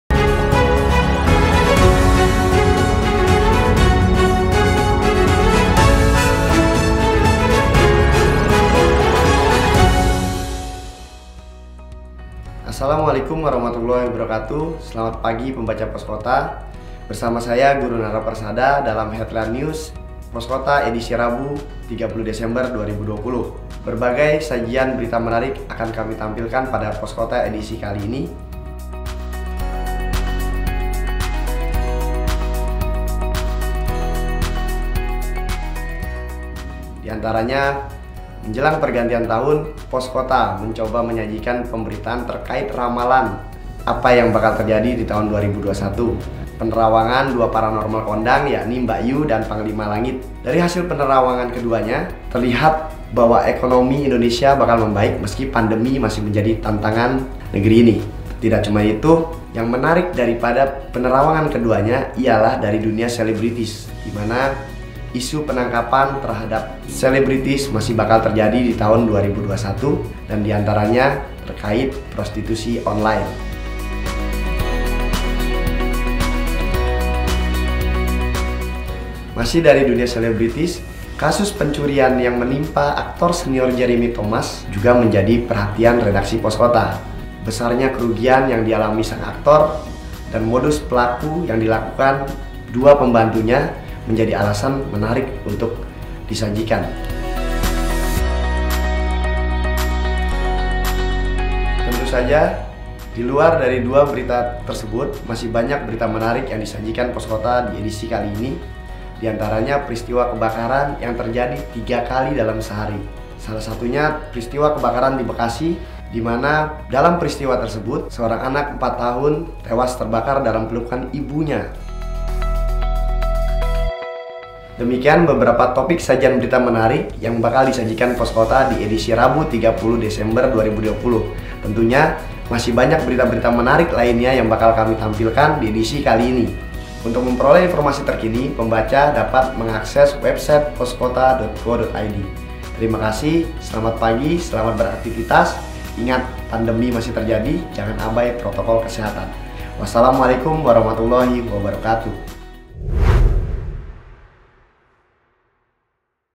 HEADLINE HARIAN POS KOTA EDISI RABU, 30 DESEMBER 2020
Harian Pos Kota menyajikan beberapa topik berita menarik seputar Jabodetabek, mulai ramalan peristiwa yang bakal terjadi di tahun 2021hasil penerawangan dua paranormal, PRT menguras harta aktor Jeremy Thomas, status terbaru video syur artis Gisel, hingga peristiwa kebakaran yang menewaskan ibu dan anak.
Juga berita-berita nasional, metropolitan, bodetabek, sport dan showbiz.